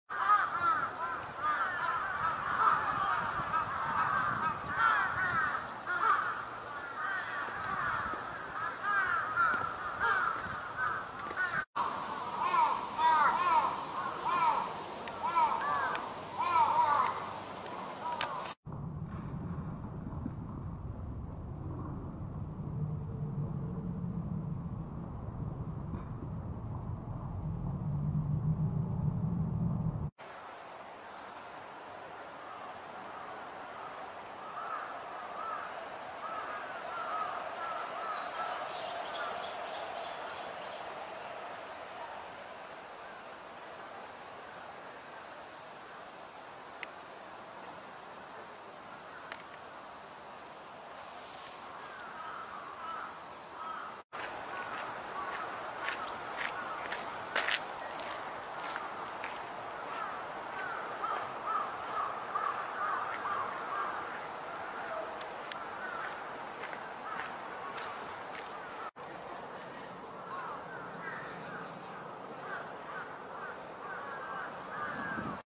字幕誤有⁂自然教育園Part4～Institute for Nature Study, National Science Museum
⁂The first reptile is the Takydromus tachydromoides.最初の爬虫類はニホンカナヘビです⁂

ヨダンハエトリ♂Marpissa pulla
イヌビワハマキモドキChoreutis japonica
オオナミモンマダラハマキCharitographa mikadonis
オオシオカラトンボOrthetrum triangulare melania